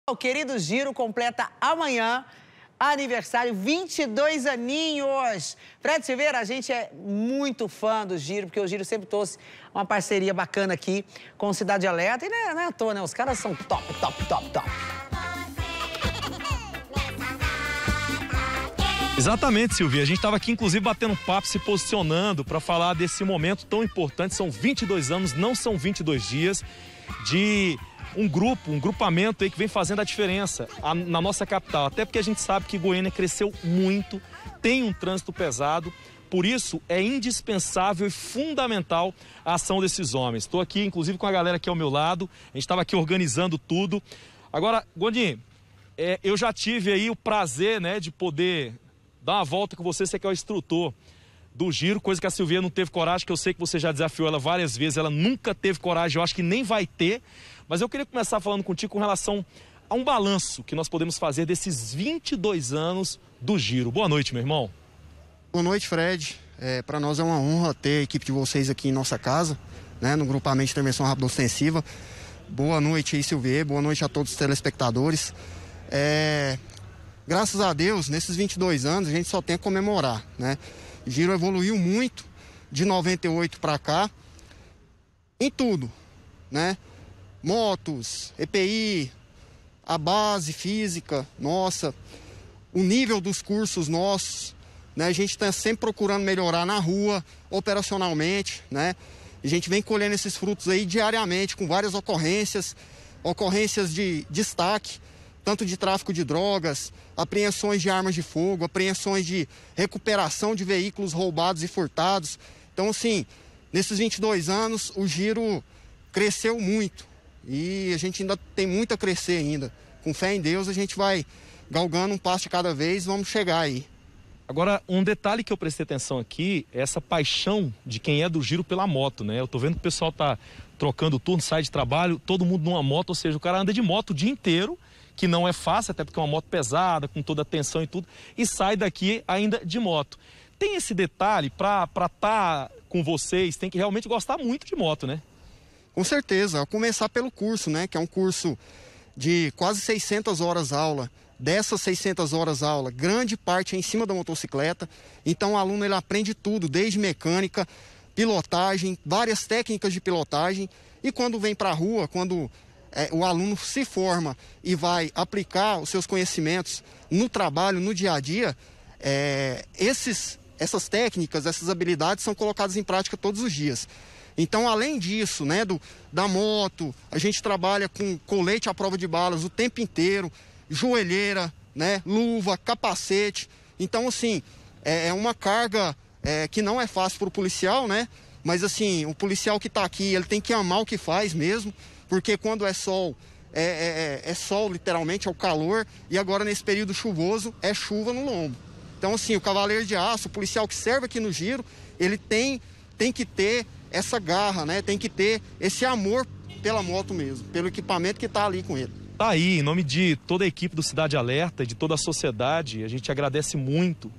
PARABÉNS GIRO COMPLETA 22 ANOS
O GRUPAMENTO DE INTERVENÇÃO RÁPIDA OSTENSIVA. O QUERIDO GIRO, COMPLETA AMANHÃ 22 ANOS.